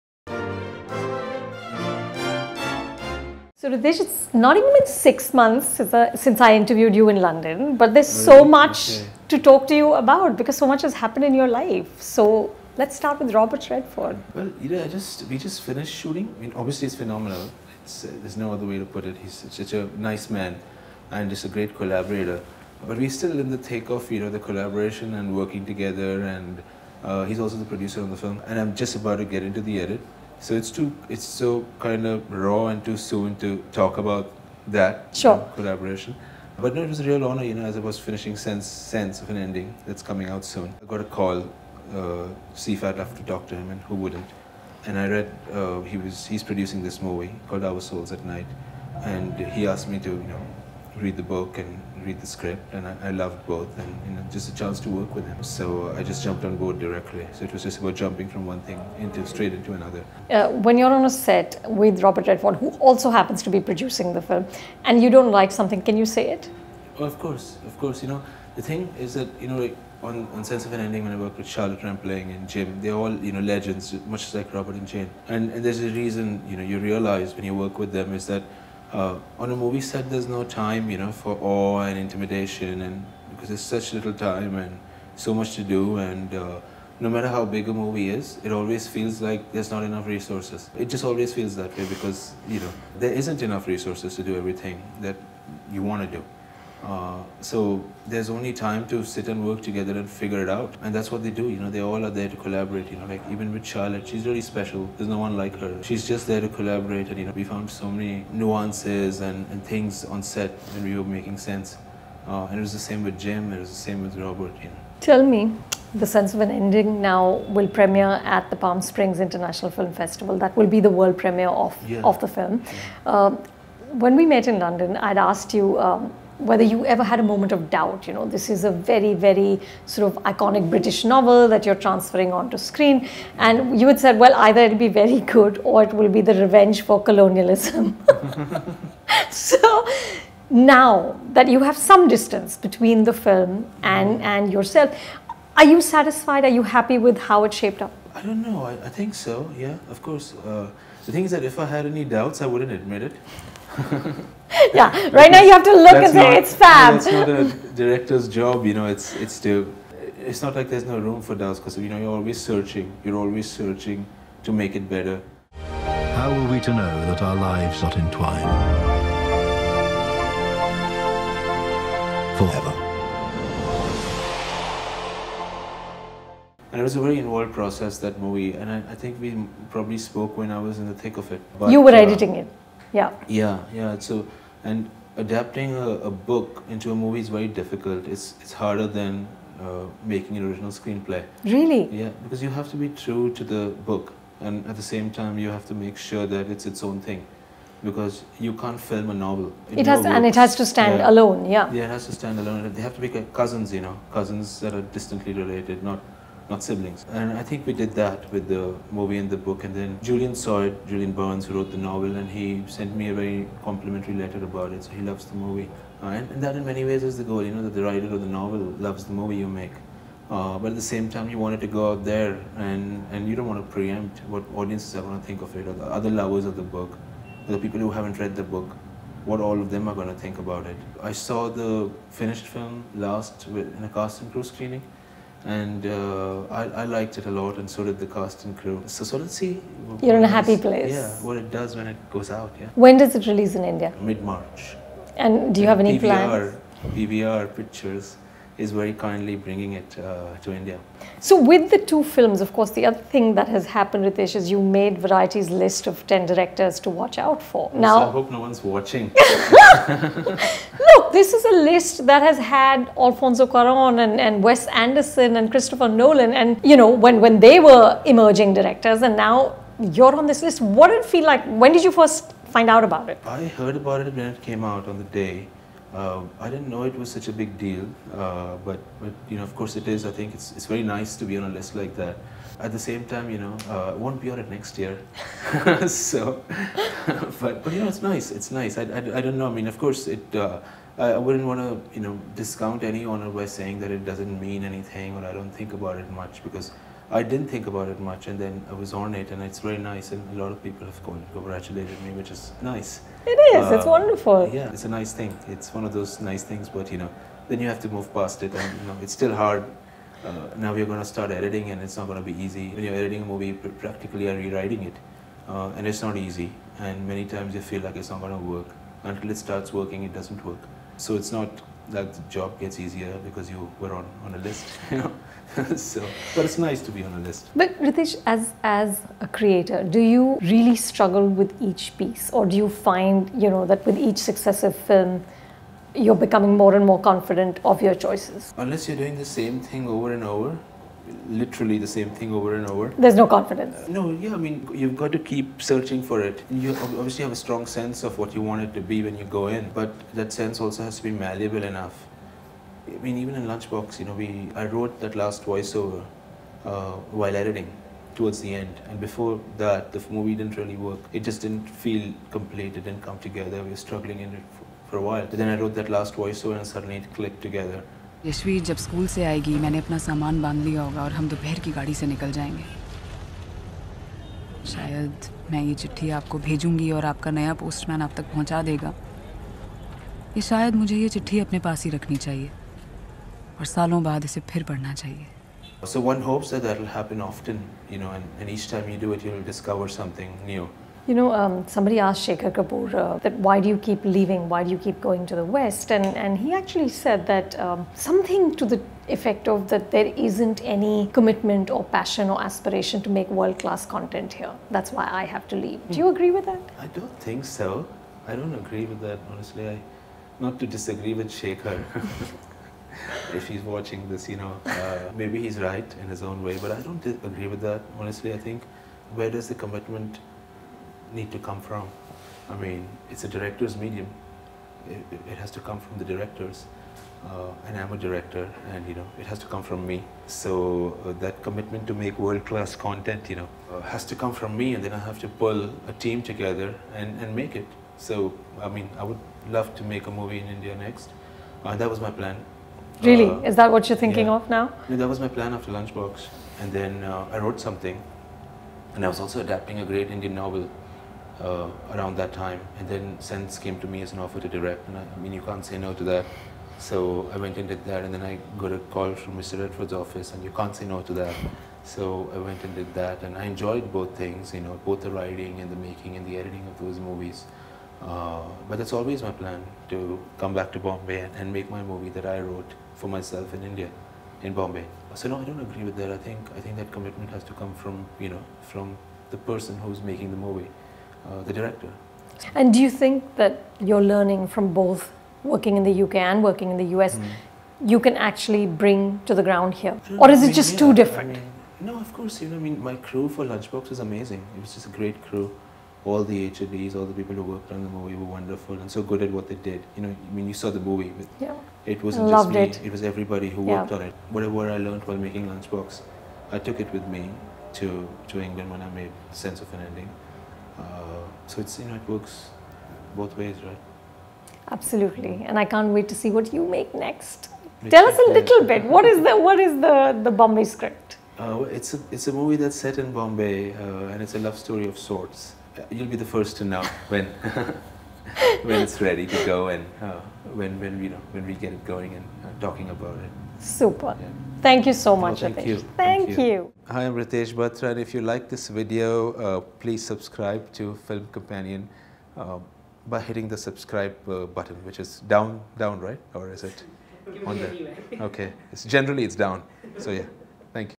Ritesh Batra Interview | Anupama Chopra | Face Time
Ritesh Batra's had a fabulous year. His film The Sense of an Ending, an adaptation of a Booker Prize-winning book by Julian Barnes, is up for release. He's currently filming a movie with Hollywood legends Robert Redford and Jane Fonda for Netflix. And he's been named in Variety's list of directors to watch out for. This list has previously featured the likes of Christopher Nolan and Wes Anderson. We catch up with The Lunchbox director on all these exciting developments.